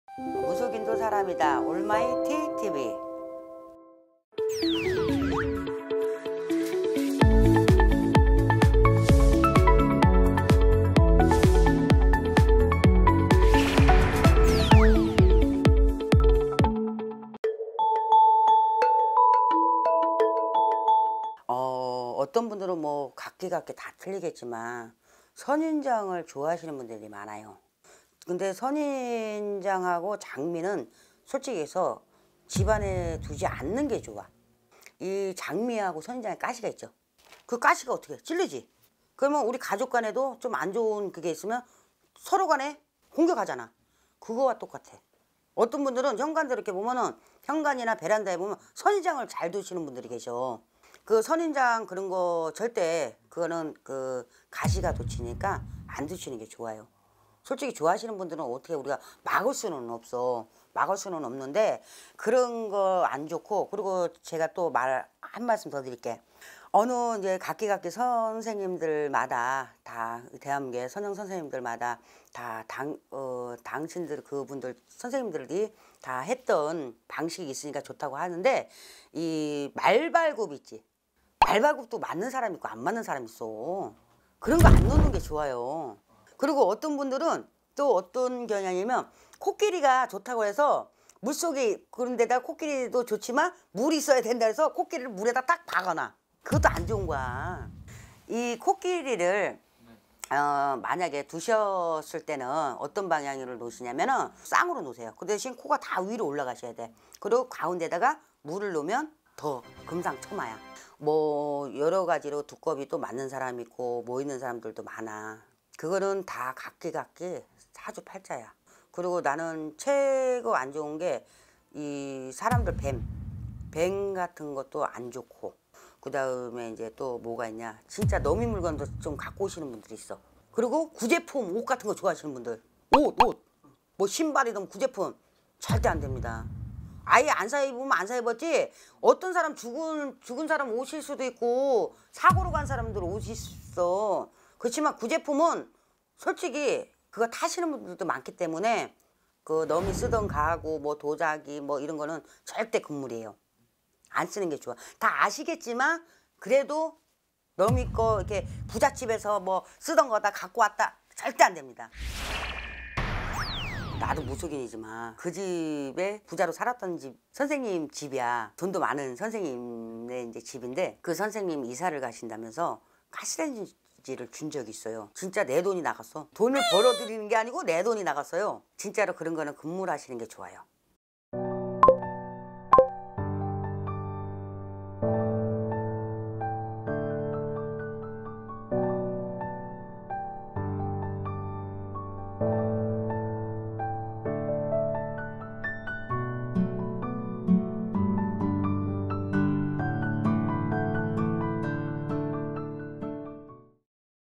2020년 잘살고싶으신가요? xx물건들은 집에서 당장 버리세요 성남점집 동해용신장군
자막

★성남점집 동해용신장군★

★대표블로그★
-올마이티TV